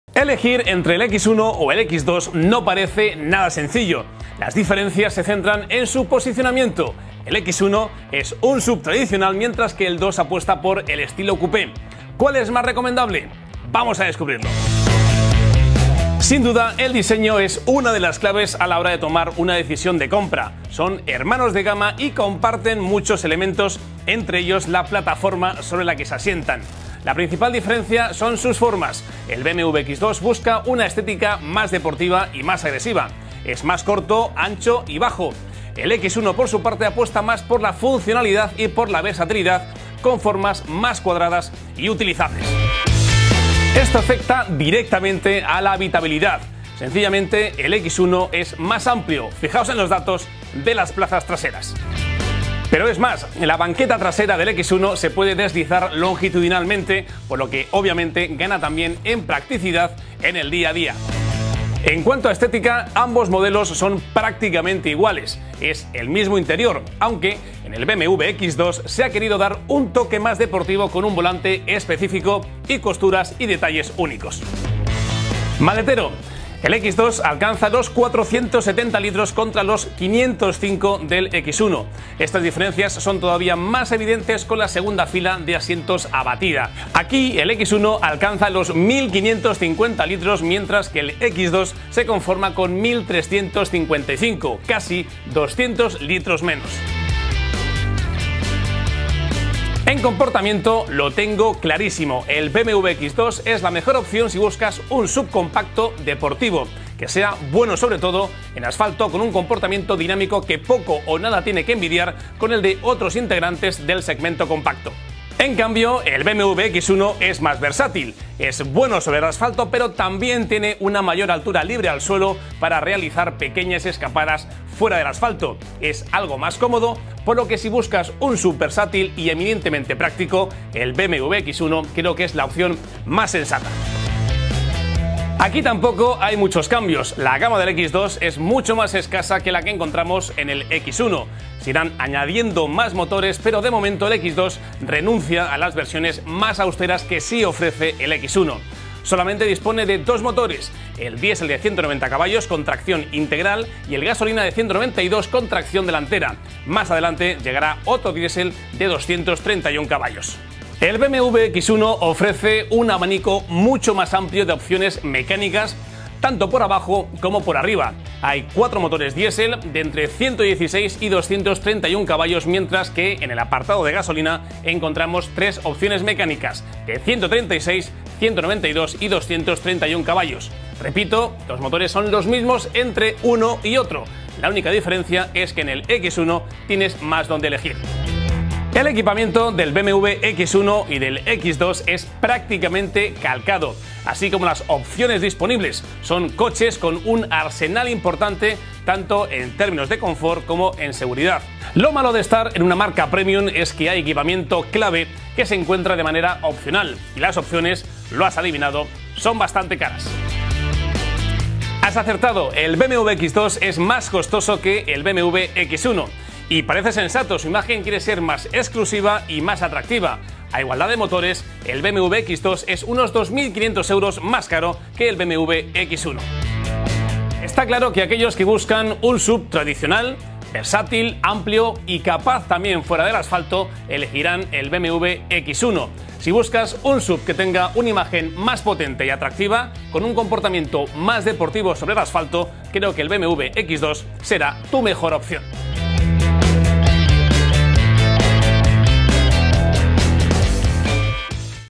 Cara a cara BMW X1 vs BMW X2, ¿cuál comprar?
Vamos a analizar en este vídeo dos modelos de BMW que están dando mucho que hablar. Cara a cara al detalle entre el BMW X2 y el BMW X1. ¿Cuál conviene comprar? Aquí lo analizamos.

El BMW X1 es el hermano del BMW X2 ya que éste es, básicamente, su variante coupé. A continuación vamos a comprobar cuánto del X1 se esconde en este X2.